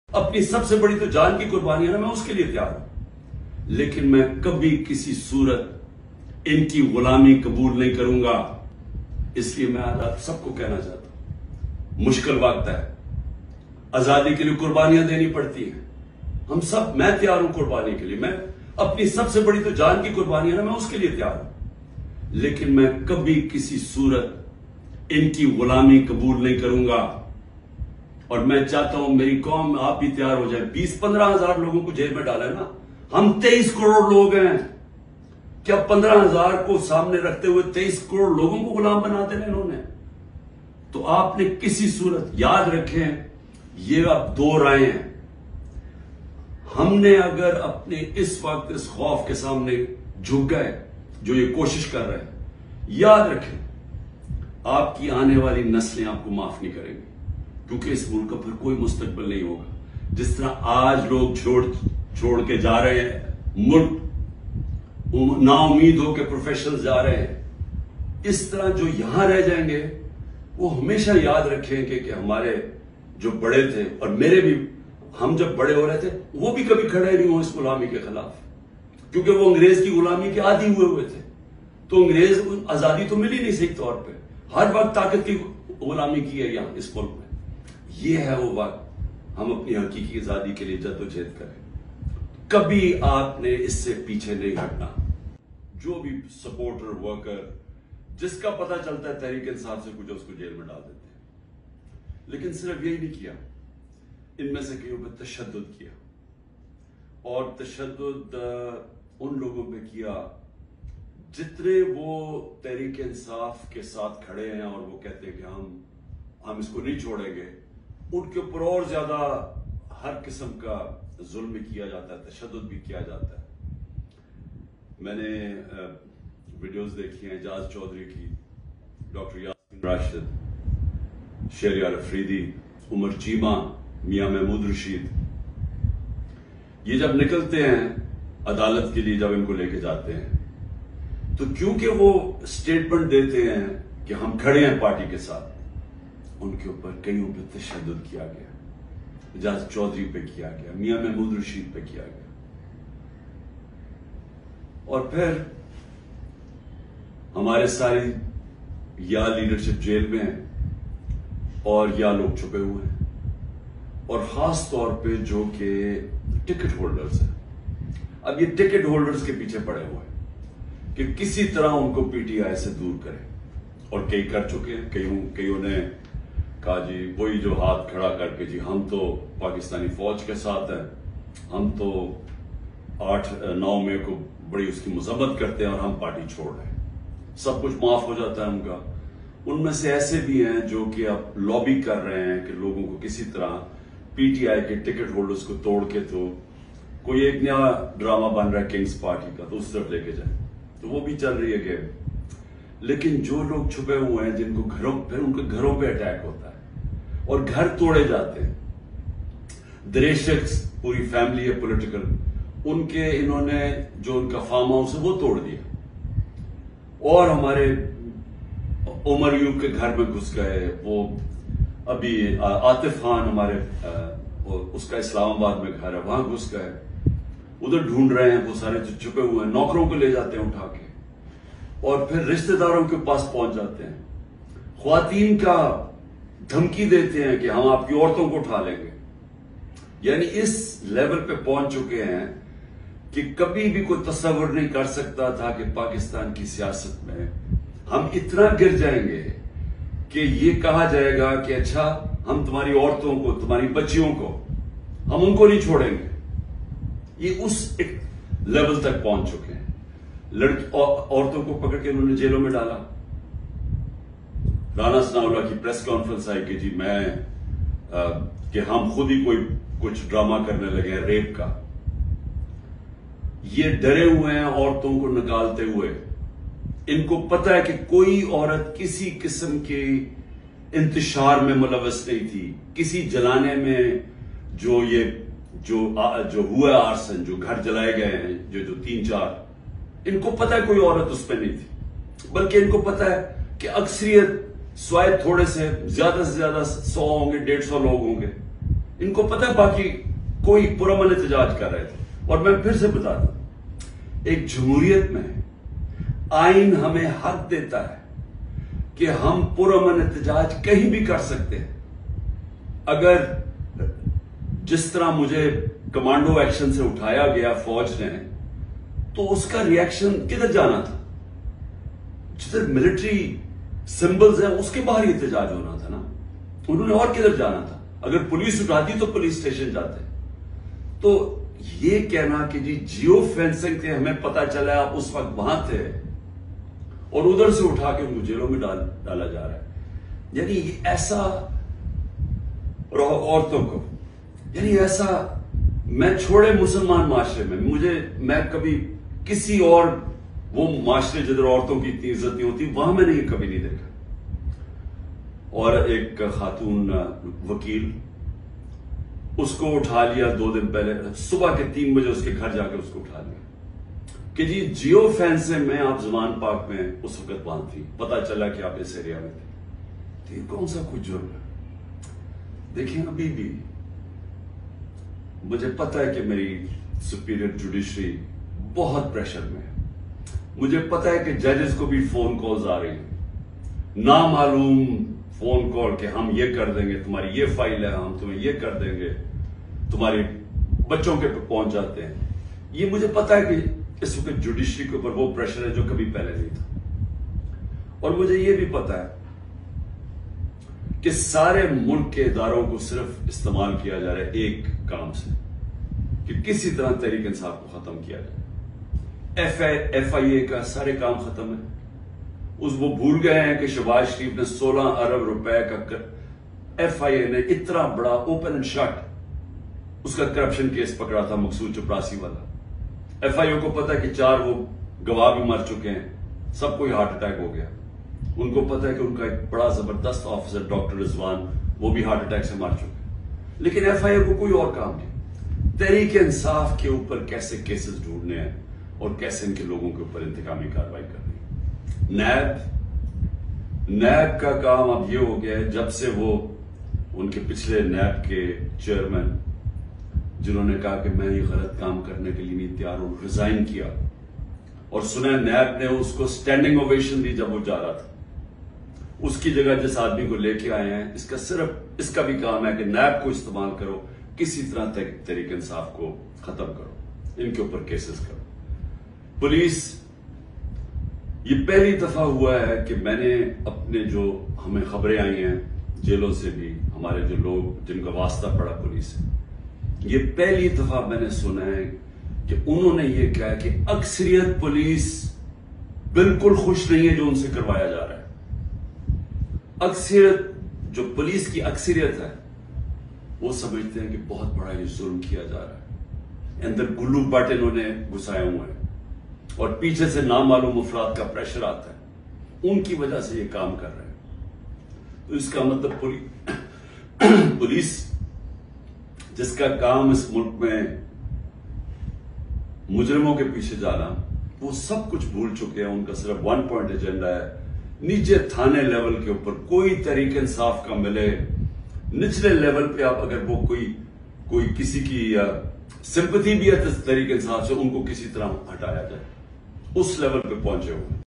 Imran khan today’s address to Nation |پاکستان کیلے جان بھی دے دونگا غلامی نہیں کرونگا
عمران خان نہیں جھک سکے کہہ دیا غلامی سے موت بہتر ہے